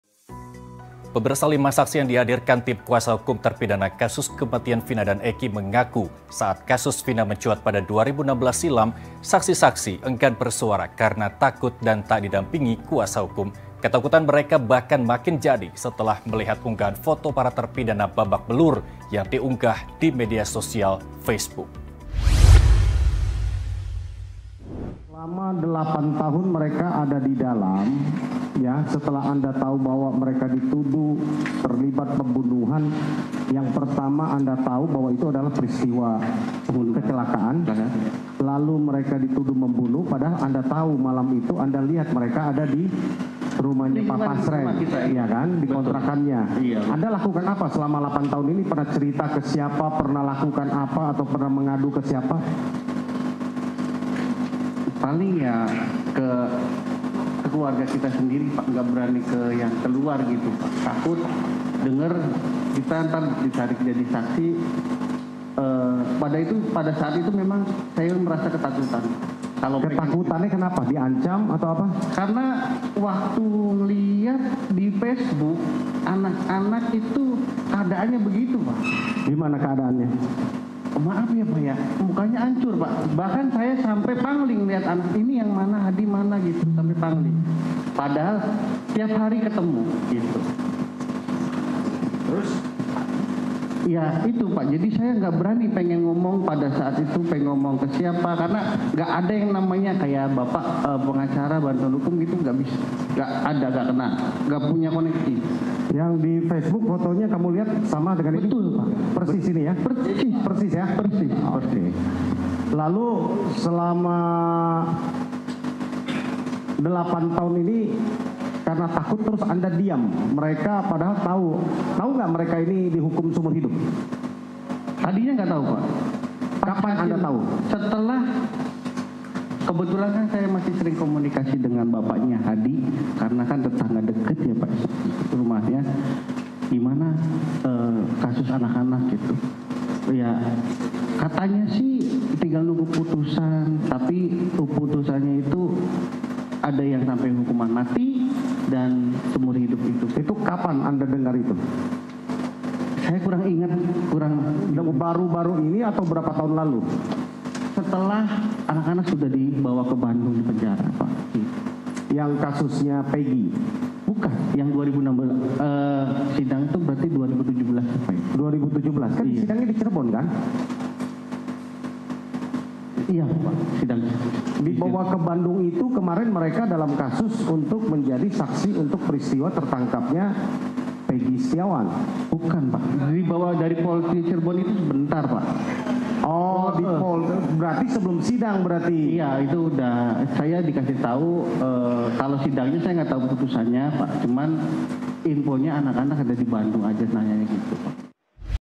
Saksi PK Kasus Vina, Darmanto: Takut karena Foto Brutal, Sampai Pangling Ngelihatnya | HIGHLIGHTS
Mereka baru-baru ini memutuskan untuk berbicara demi mencari keadilan setelah 8 tahun ketakutan.

DYFNTV